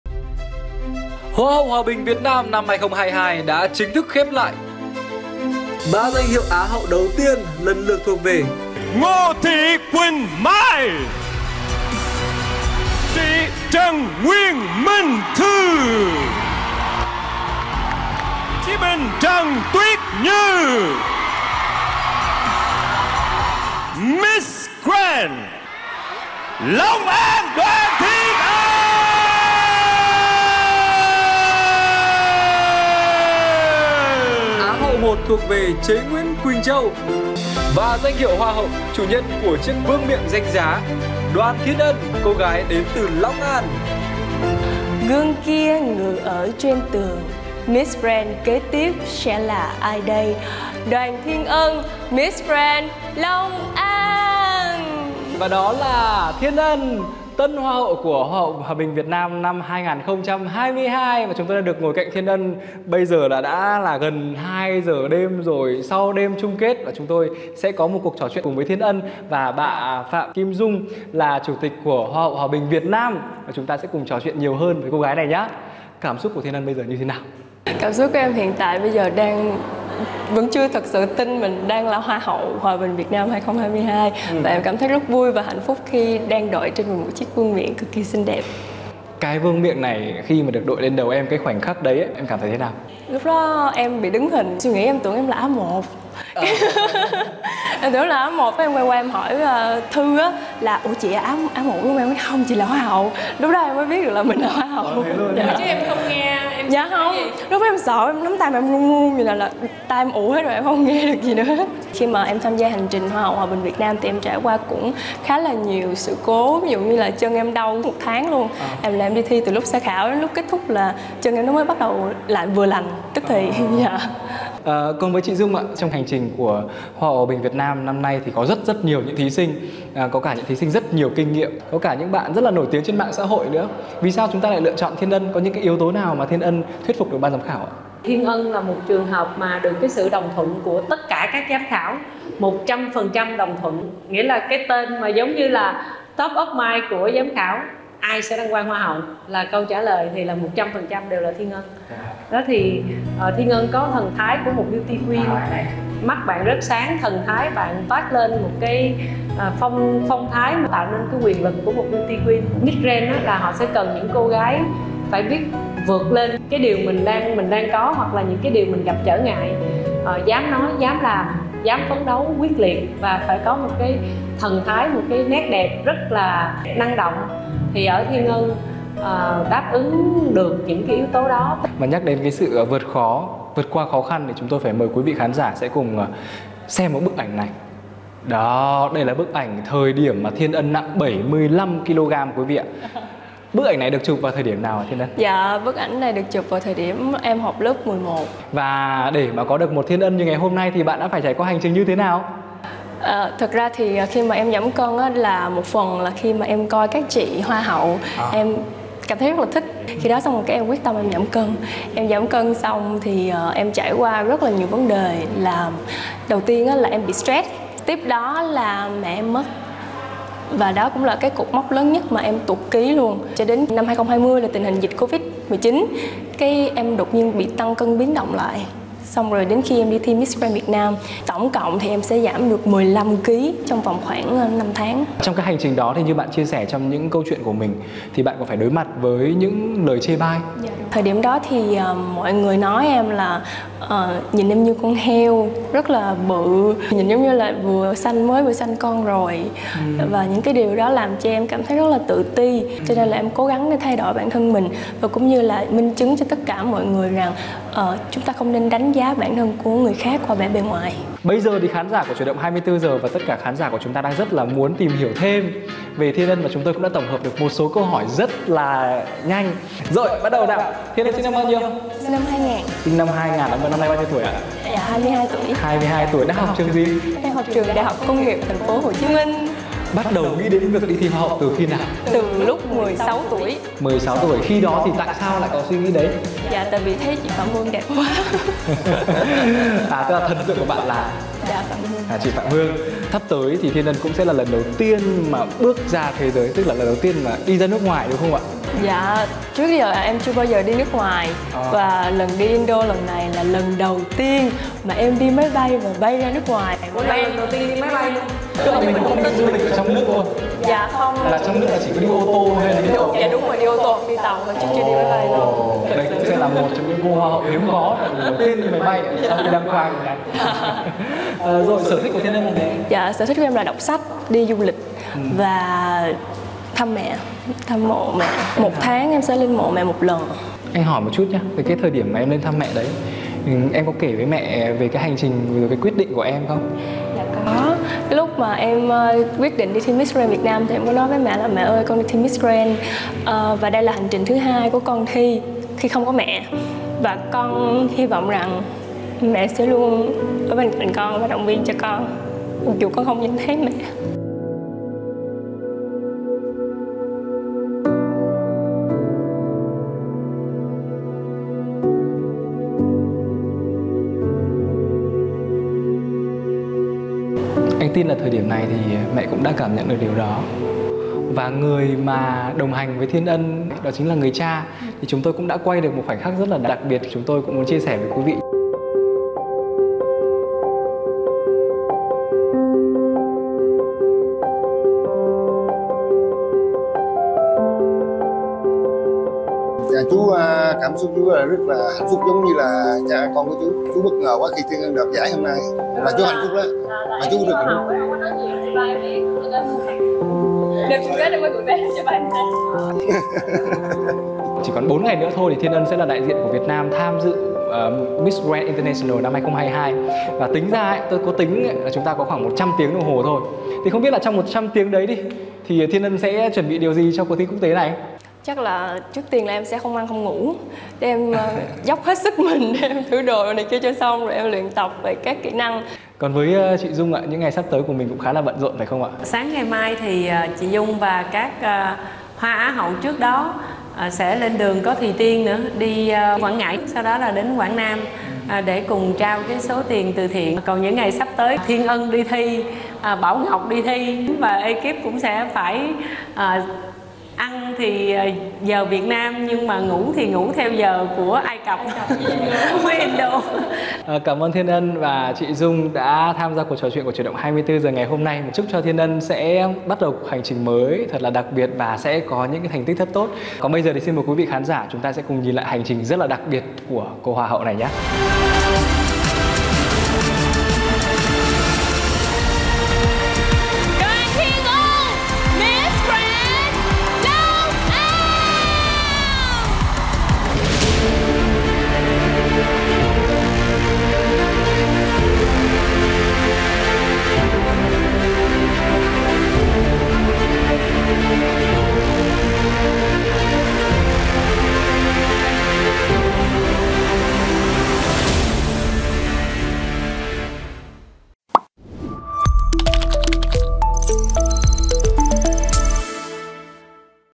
Đoàn Thiên Ân và hành trình trở thành Hoa hậu Hòa bình Việt Nam 2022 | VTV24
Trong phần thuyết trình, Tân Hoa hậu Hoà bình VN Thiên Ân chia sẻ từng bị miệt thị ngoại hình vì có lúc cân nặng lên đến 75kg.